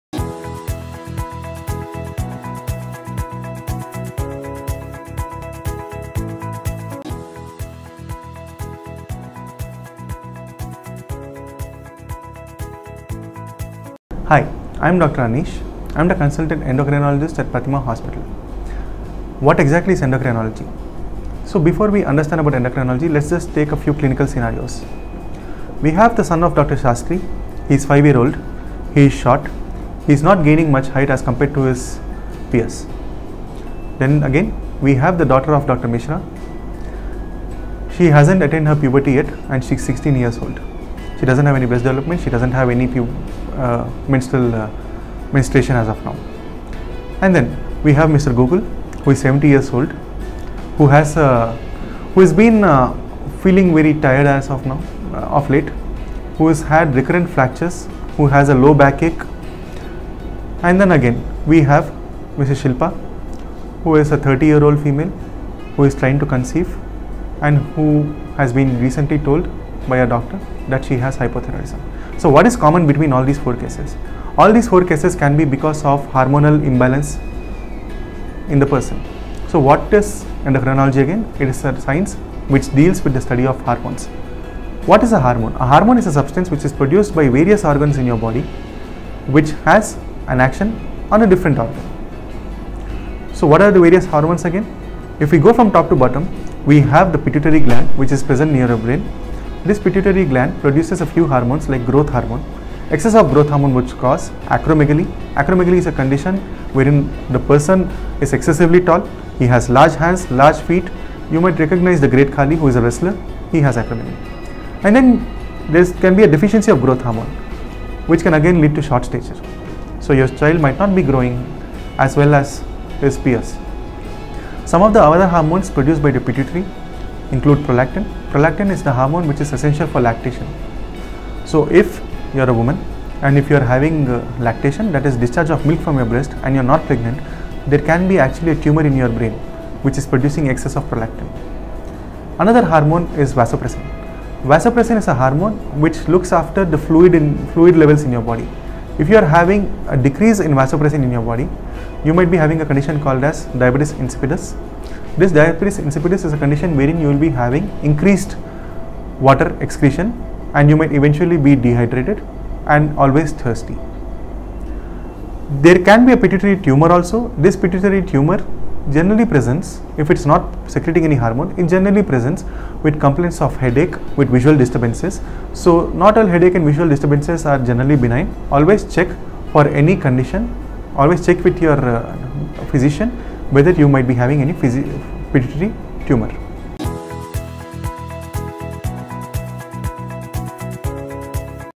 Prathima Hospitals: Department of Endocrinology | Treatment and Services
"Department of Endocrinology at Prathima Hospitals.
Endocrinology is the study of the various hormones and their disorders in the body. Hormone is made by organs known as glands. Hormones are substances that help to control activities in the body and have several effects on the growth and development, sexual growth and function, metabolism, reproduction, bone health, food absorption and utilization etc.

Some of the glands that make up the endocrine system include the pancreas, thyroid, pituitary, hypothalamus, parathyroids, adrenals, ovaries, testes and pineal gland."